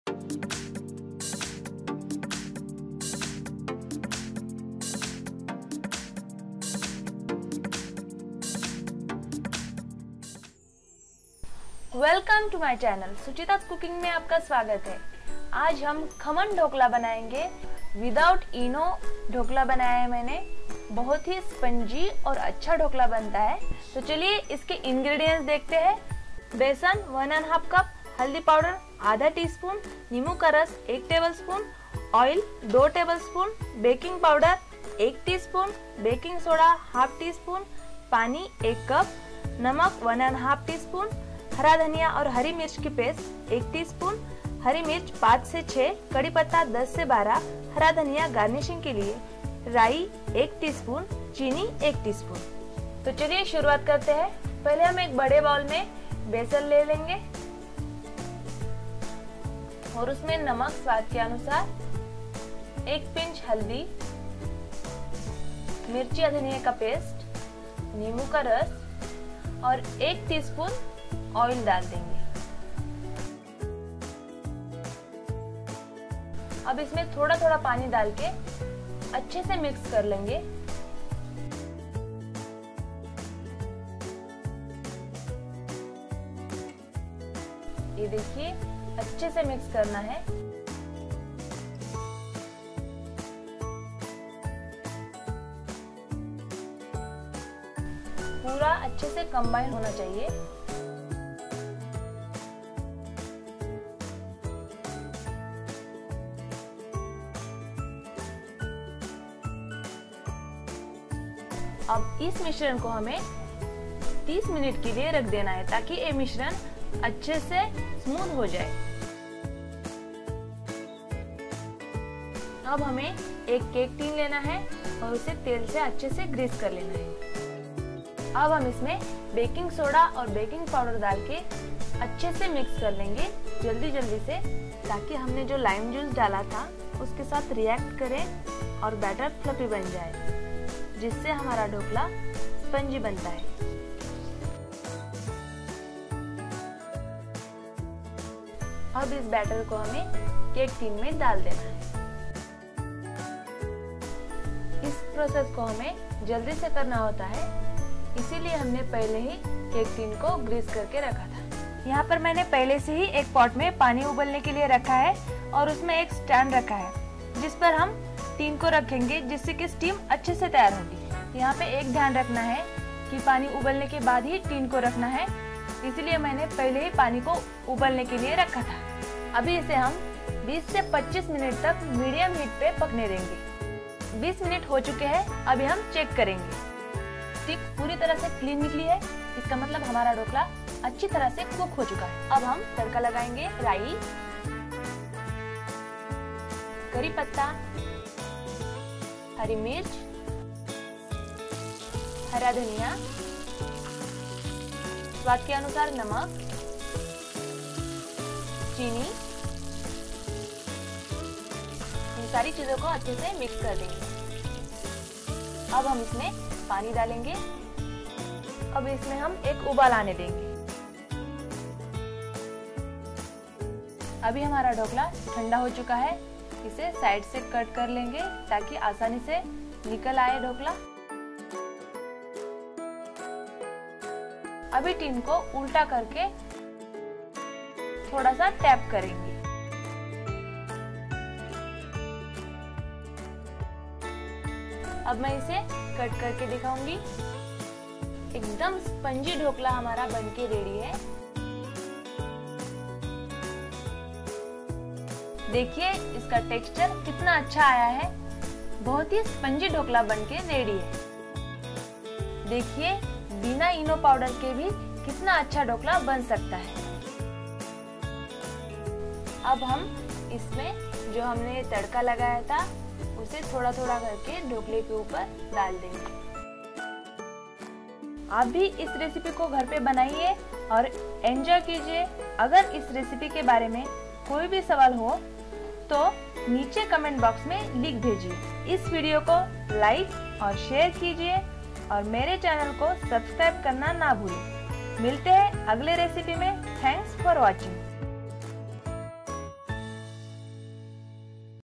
SPONGY DHOKLA (WITHOUT ENO)/Restaurant type spongy dhokla recipe/बिना ईनो के बनाए बाज़ार जैसा ढोकला
Dhokla is a vegetarian culinary dish that is found mainly in the Indian state of Gujarat and parts of adjacent states. It is made with a fermented batter derived from rice, ground urad dal, and chickpea flour. Dhokla can be eaten for breakfast, as a main course, as a side dish, or as a snack. Dhokla is very similar to Khaman, however Dhokla is made of batter derived from mixture of rice flour and chickpea flour, whereas Khaman is typically made from Chickpeas gram and looks yellow in color
DHOKLA RECIPE
Khaman dhokla
Khaman dhokla Recipe in Hindi
dhokla recipe without eno
fluffy dhokla recipe

-:INGREDIENTS:-
: GRAM FLOUR 1  1/2CUP
: OIL 2TBSP
: WATER 1CUP
: SALT 1 1/2TSP
: CORIANDER, GREEN CHILLI PASTE 1TSP
: GREEN CHILLI 5-6: